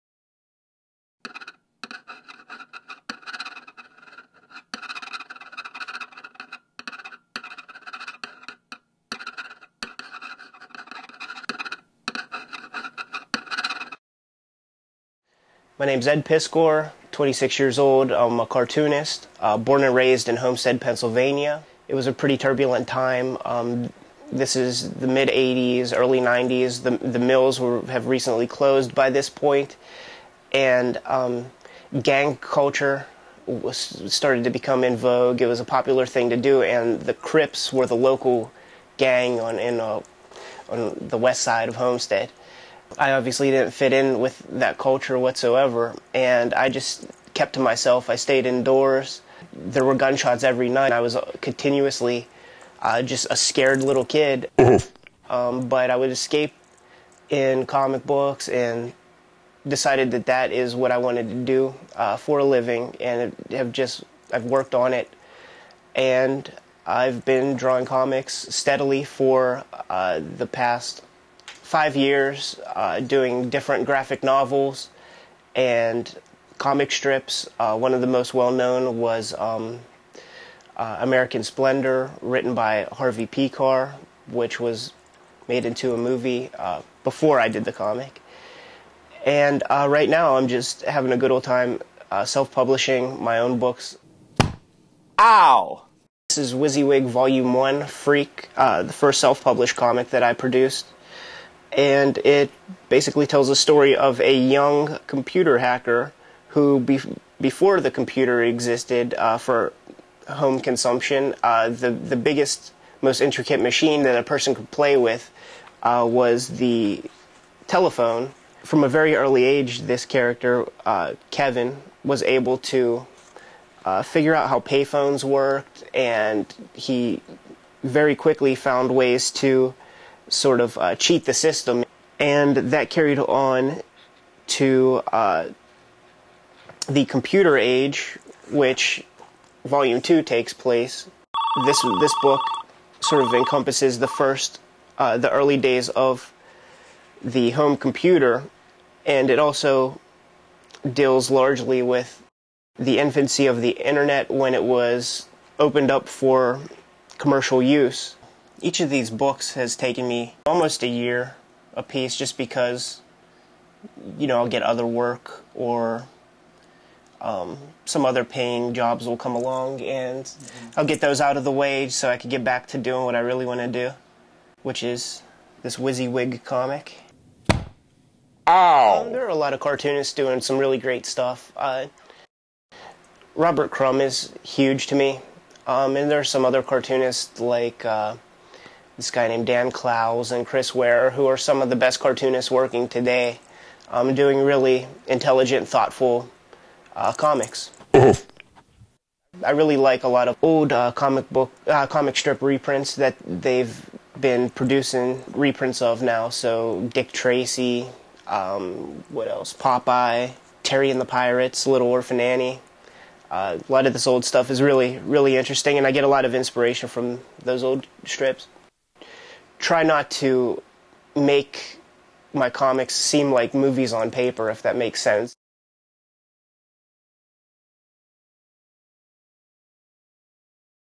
Ed Piskor, cartoonist
Ed Piskor, a 26-year-old cartoonist and Pittsburgher, has published two graphic novels, including "Wizzywig," about 1980s hackers.